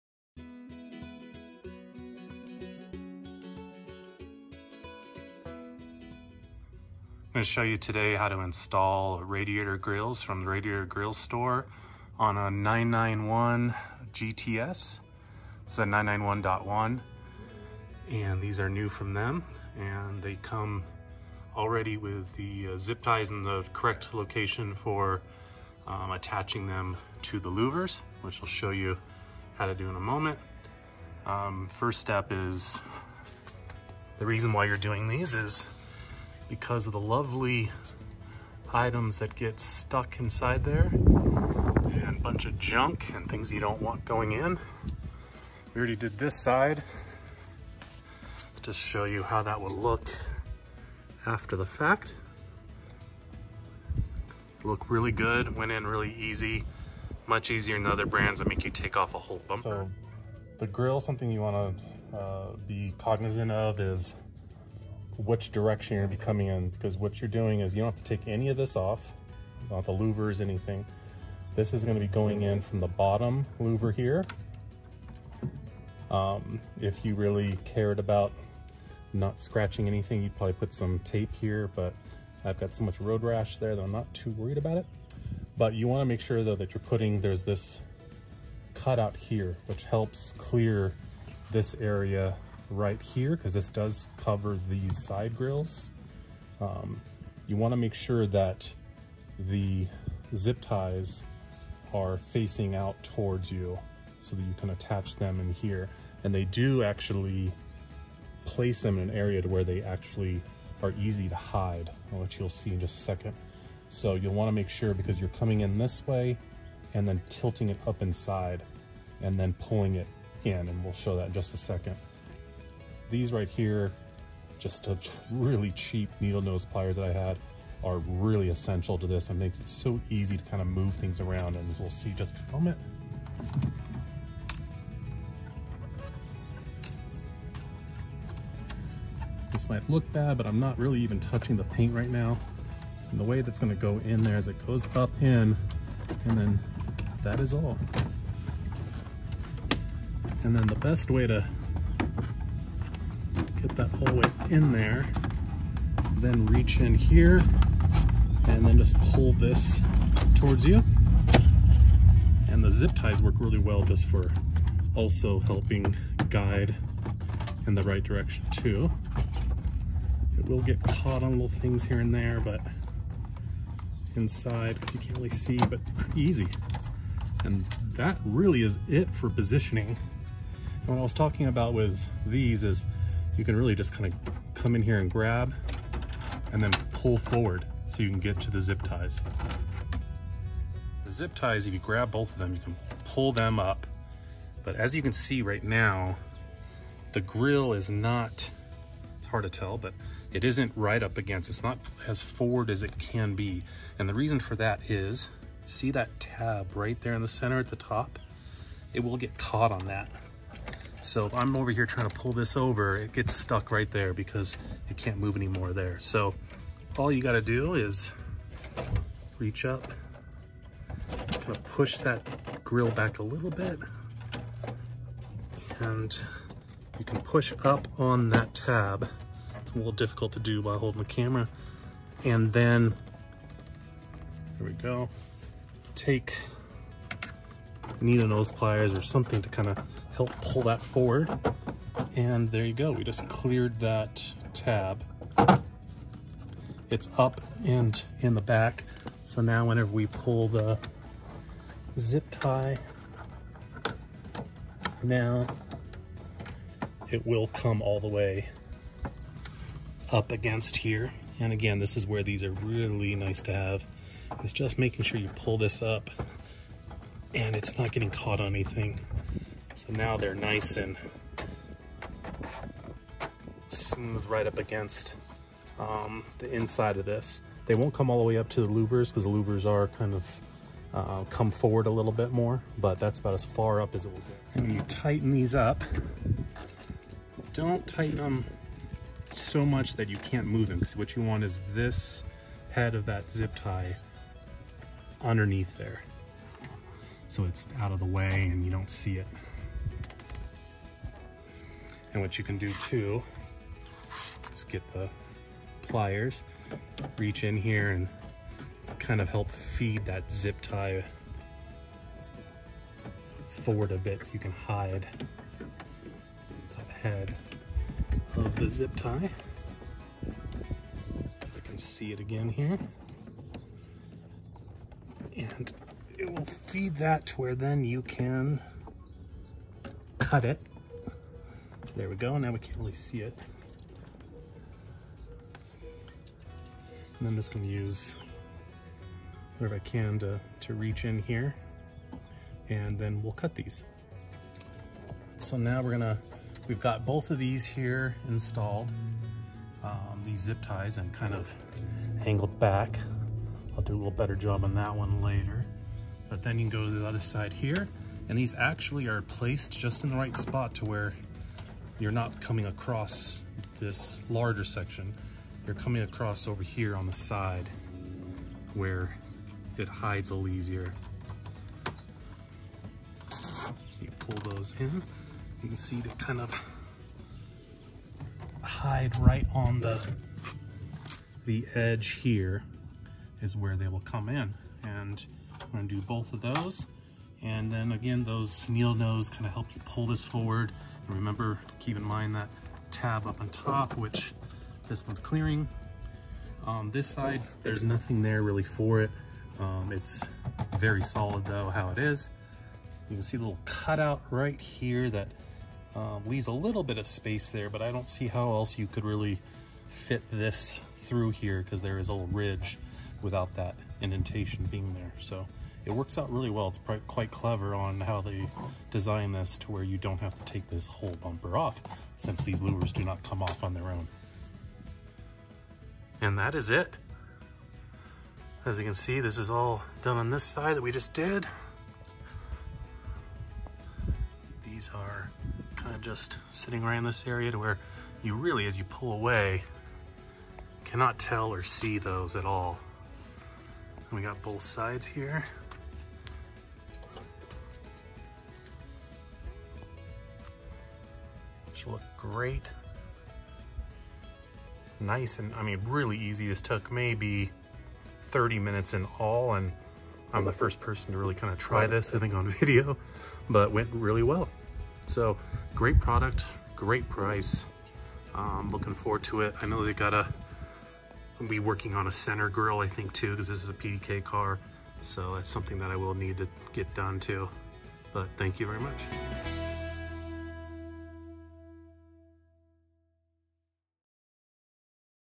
Porsche 911 991.1 GTS Radiator Grille Install by Radiator Grille Store
Radiator Grille Store is pleased to present the front left and right Radiator Grille‘s for the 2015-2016 Porsche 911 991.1 GTS. These radiator grilles install without bumper removal and in minutes with basic hand tools. Protect your vulnerable condensers and radiators with this simple modification. Create an OEM looking, aggressive look with protection.